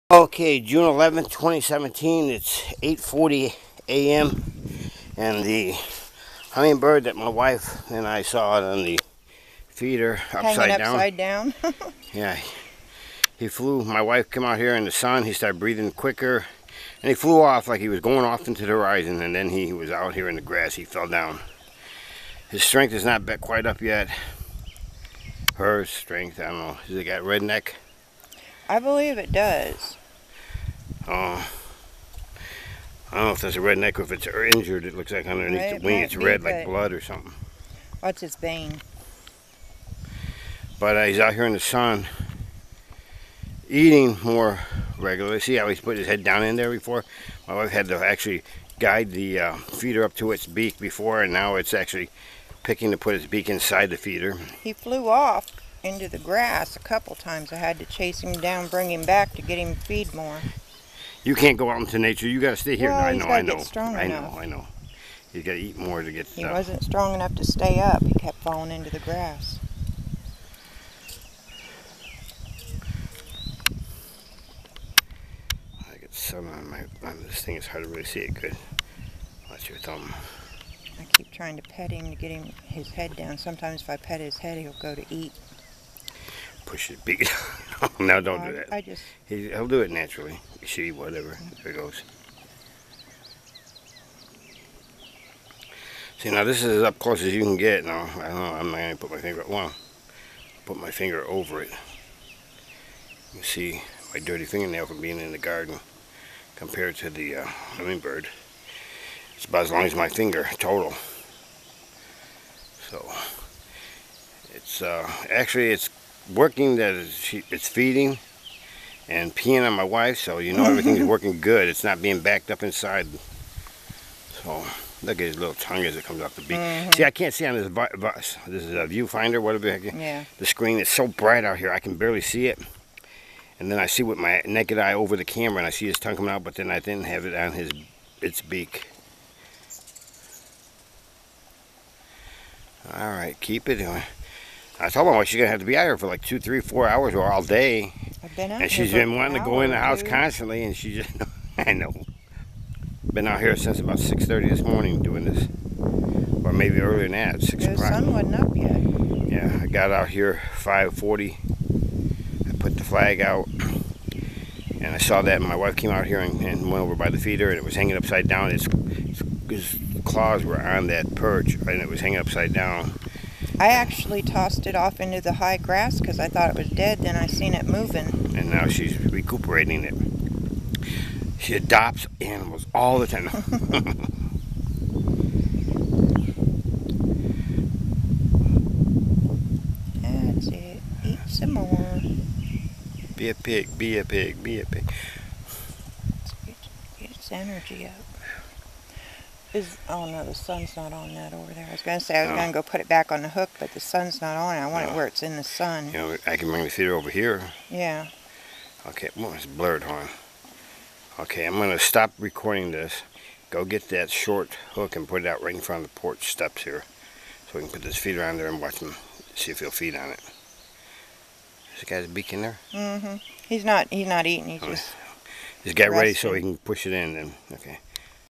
WITH MINOR HUMAN INTERVENTION WITH LOVE PART 2
With my WIFE'S intervention, her LOVE and time.. we hope to help this LITTLE GUY/GIRL fully recover. Heating up the body and HAND feeding slightly & encouraging it to FEED on its own.